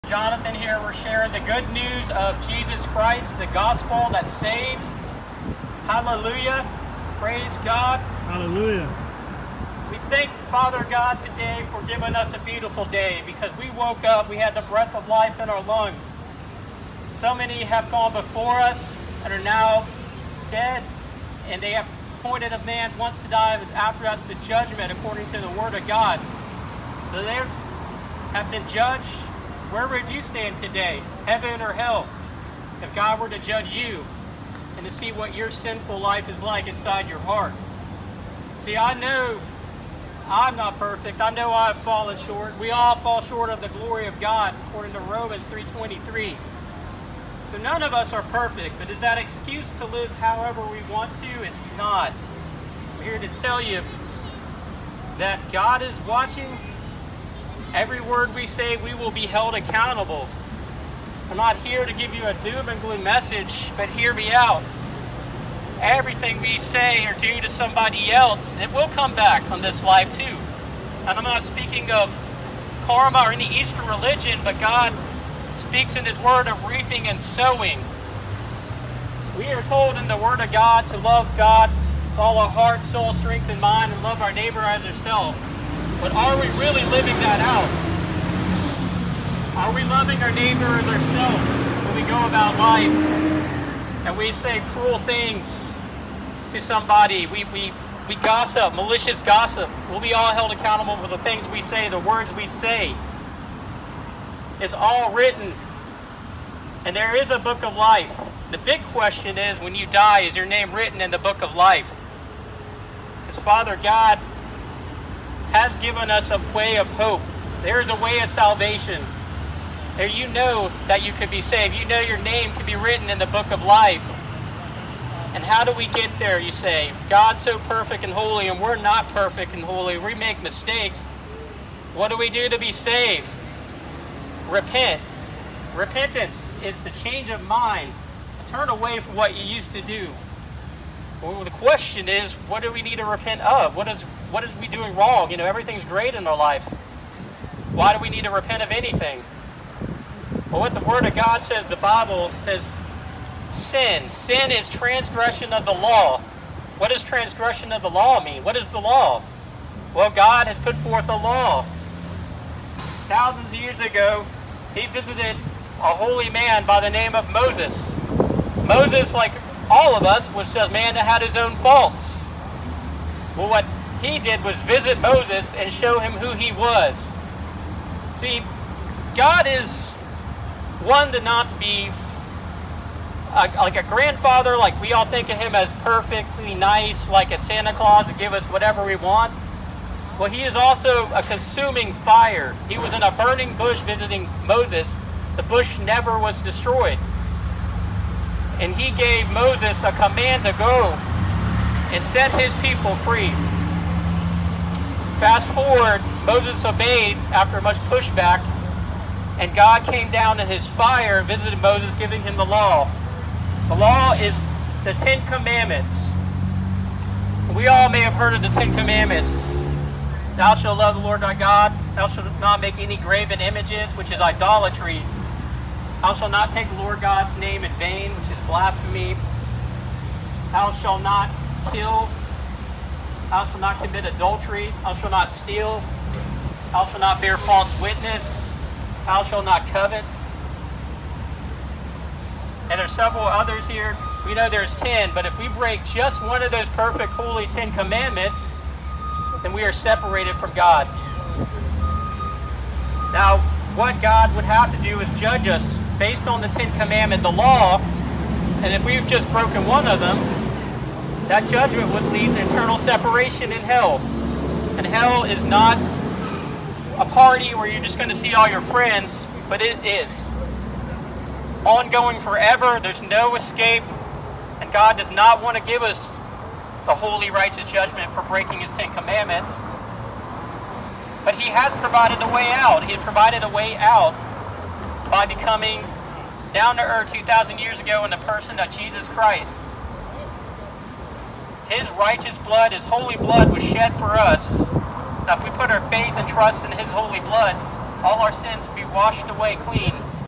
Open Air Preaching in downtown Charleston 10/10/22
Open air preaching in downtown Charleston SC on Columbus Day. Shared the gospel and several tracts were shared with many passing by.